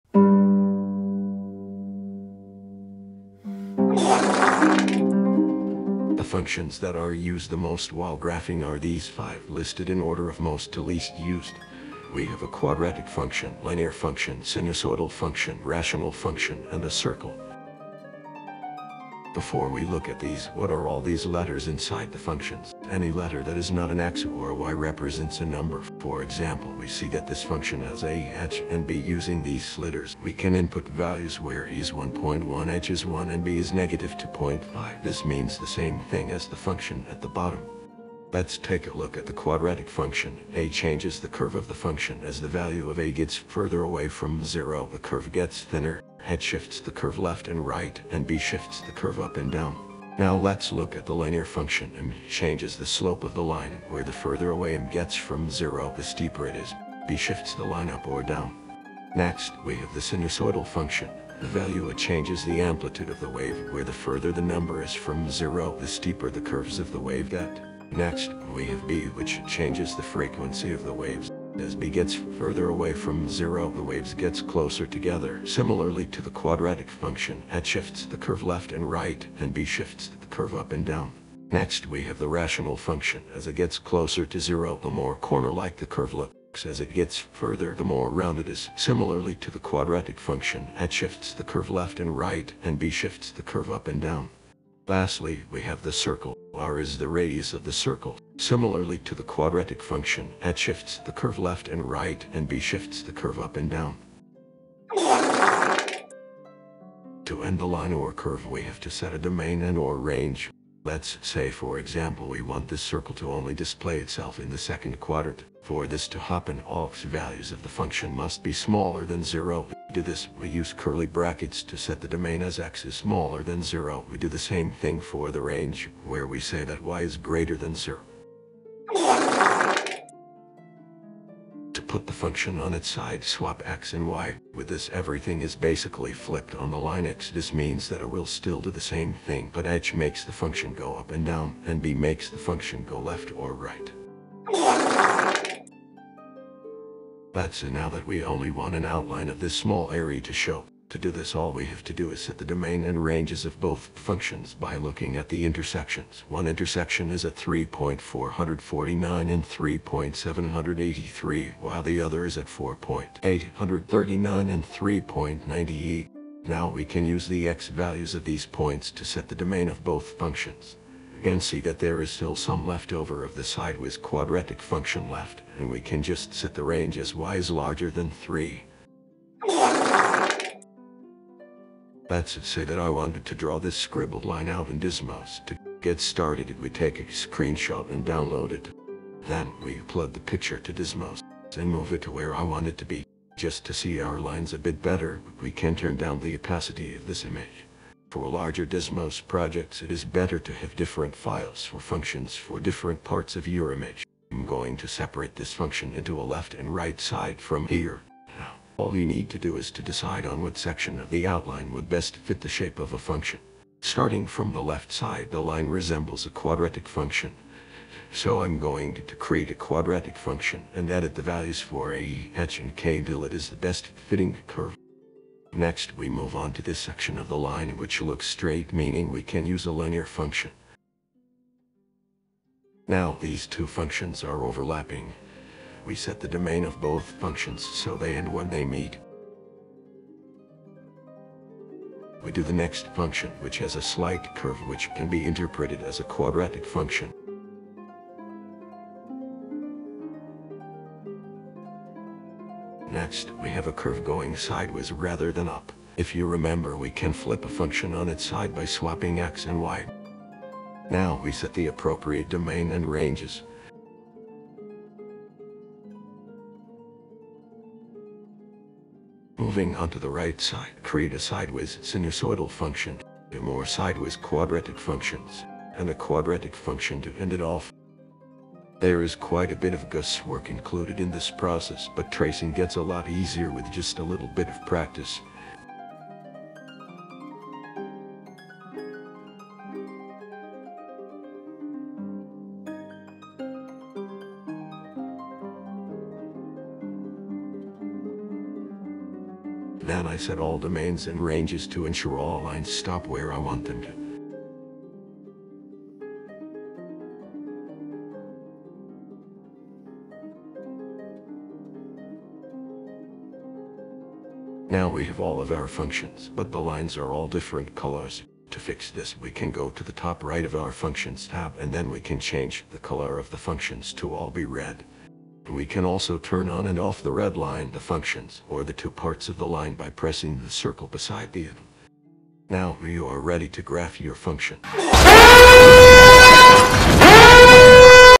Desmos Art Full Guide | Desmos Graphing Calculator (walter white is your math teacher wow)
breaking bad

how to trace/draw in desmos
hope this tutorial makes sense :))

if you want to estimate how long something will take, for reference the 8 functions took me around 14 minutes.

Chapters:
0:03 functions
1:45 ending the function
2:08 putting the functions on its side
2:24 putting the functions together
2:59 tracing
5:24 yea